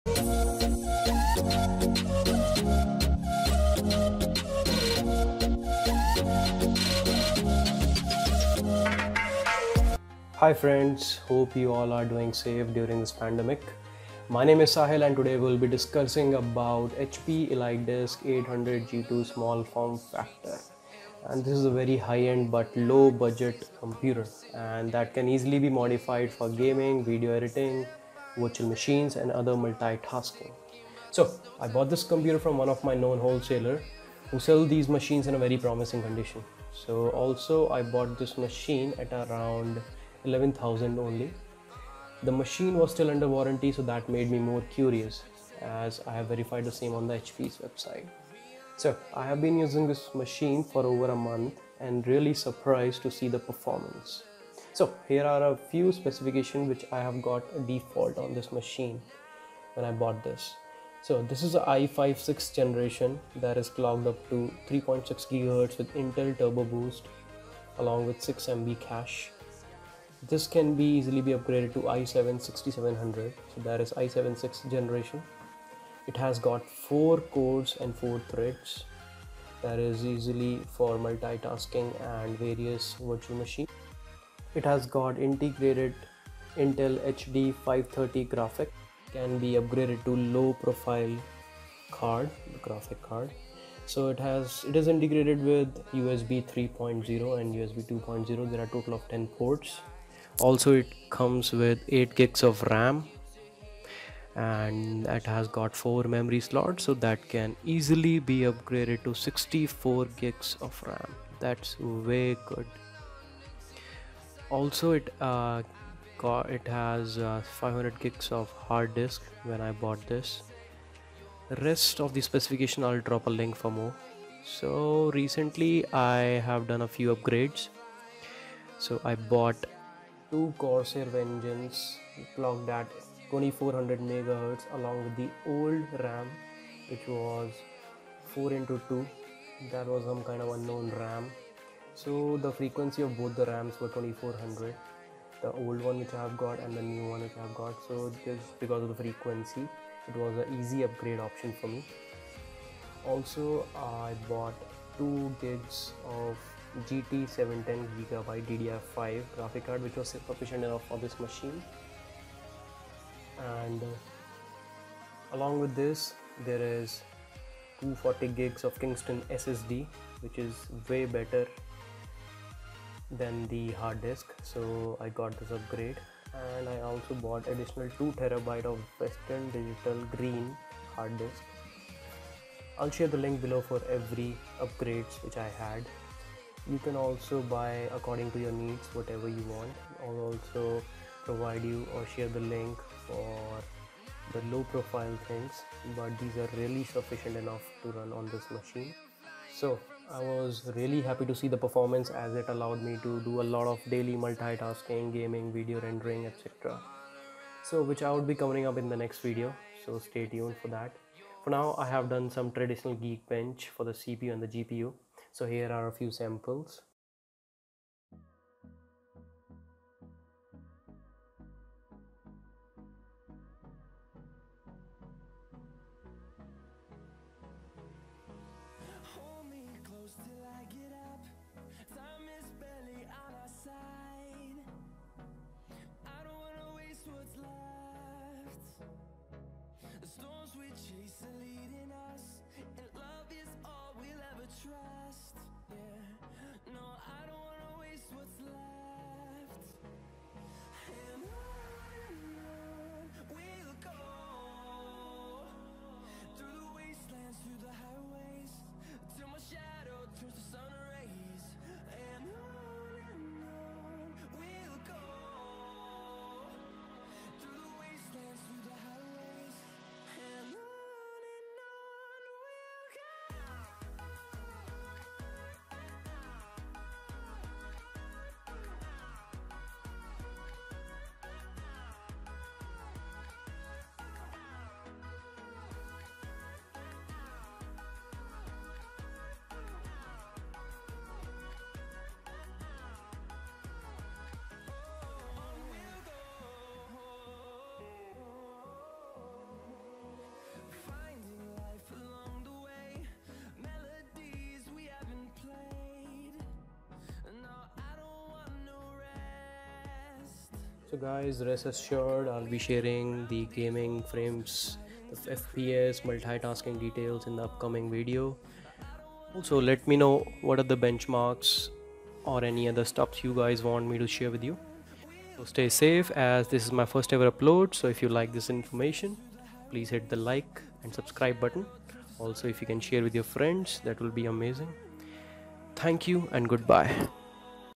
Best budget desktop computer 2020 - HP ELITEDESK 800 G2 - MUST WATCH
Threshold upgrade & Review for HP ELITEDESK 800 G2.

My Upgrade (NEW) RAM : 8X2 CORSAIR VENGENCE CLOCKED AT 2400MHZ along with the old 2400mhz 4X2 Unknown Ram so a total of 24GB Ram. Graphic card: Gigabyte GT 710 DDR5 PCIE8X2 SSD : KINGSTON 240GB HDD: ADDITIONAL 2TB WD GREEN MONITOR: AOC 22"
Specifications OLD HP ELITEDESK 800G2 Form Factor : Small Form Factor Operating System Windows 10 Pro V1909 Processors Intel® Core™ i5-6500 with Intel HD Graphics 530 (3.2 GHz, up to 3.6 GHz with Intel Turbo Boost, 6 MB cache, 4 cores); Chipset Intel® Q170 RAM : 8 GB Memory Slots 4 DIMM Internal Storage : 500 GB Available Graphics Integrated: Intel® HD Graphics Audio DTS Studio Sound™ line-out and line-in rear ports (3.5 mm), multi-streaming capable, internal speaker (standard) Expansion Slots 1 low-profile PCIe x16; 1 low-profile PCIe x4 (x16); 2 low-profile PCIe x1

UPGRADES
8X2 CORSAIR VENGENCE
2gb Gigabyte GT 710 DDR5  Graphic card
240 GB KINGSTON SSD
WD GREEN 2TB HARD DISK